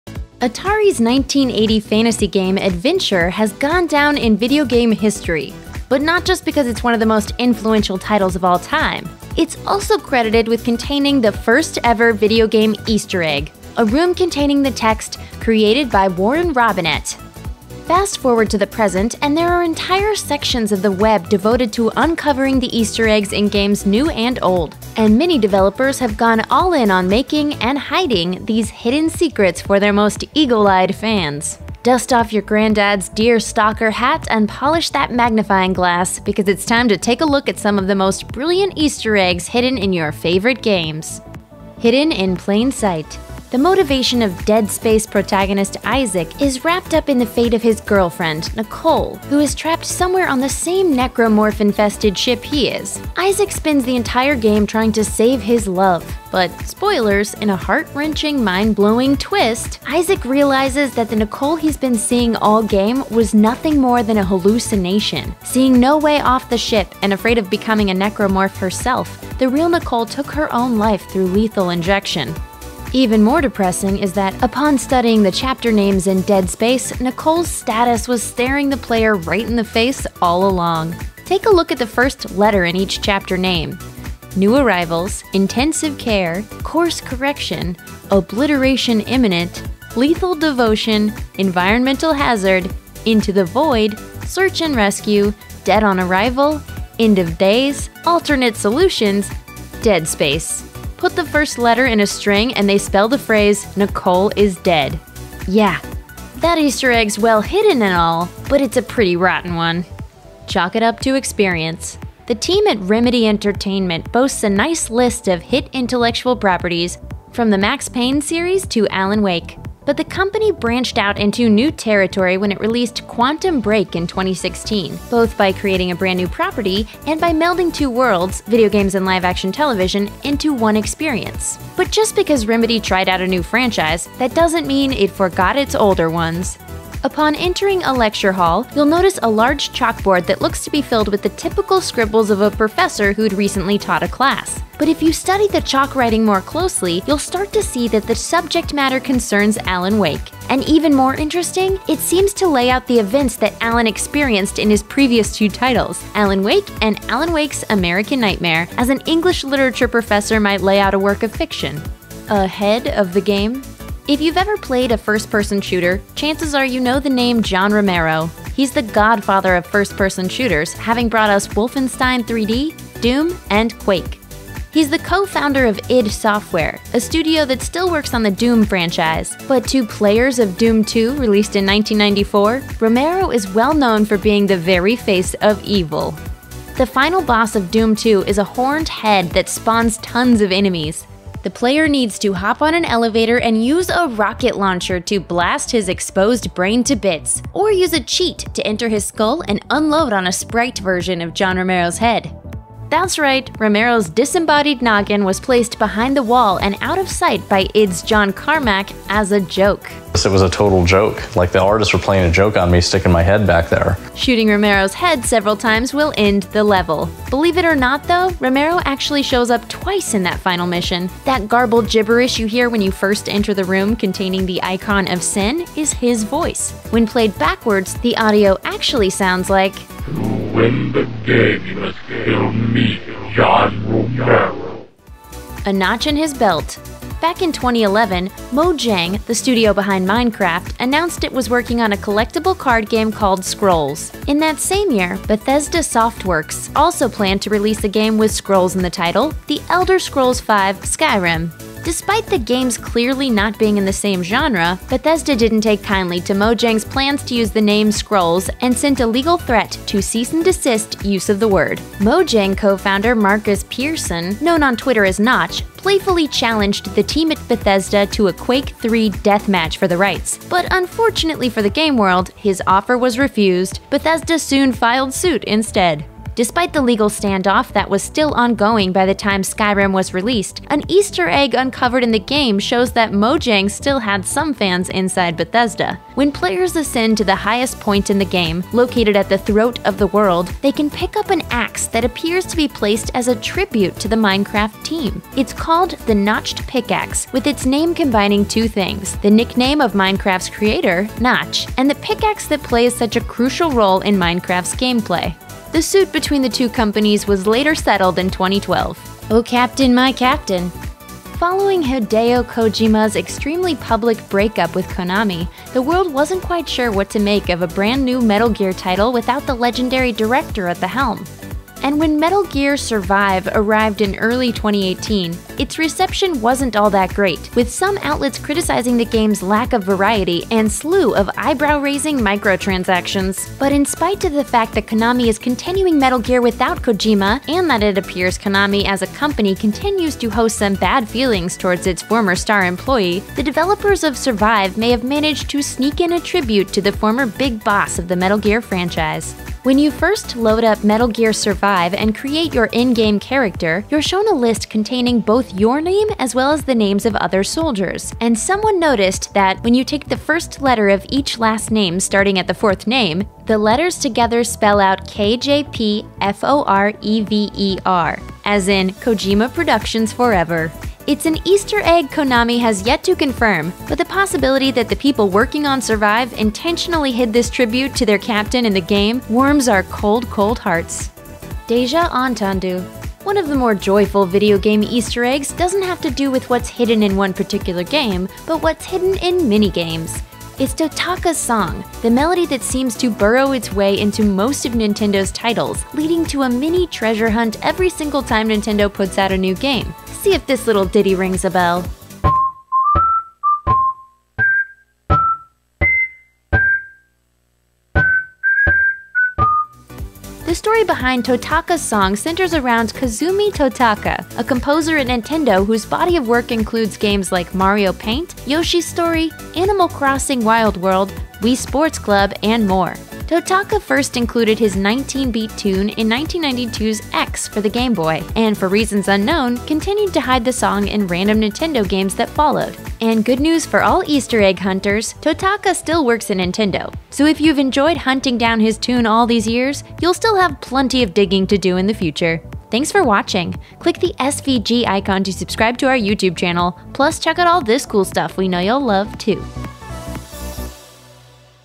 Brilliant Easter Eggs Hidden In Video Games
Developing a video game takes a lot of hours of hard work, so many developers like to sneak Easter eggs in there for the biggest gamers to find. Some are just little shout-outs to people who were crucial to the game’s existence, while others might actually help you do better in the game, but they will all give you a sense of achievement for discovering them. From the Dead Space twist that was actually revealed in its chapter titles to the surprising tribute in Metal Gear Survive, let’s take a look at some brilliant Easter eggs hidden in video games.

Hidden in plain sight | 0:00
Chalk it up to experience | 1:51
A-head of the game | 2:43
A Notch in his belt | 3:56
O Captain! My Captain! | 5:10
Deja entendu | 6:22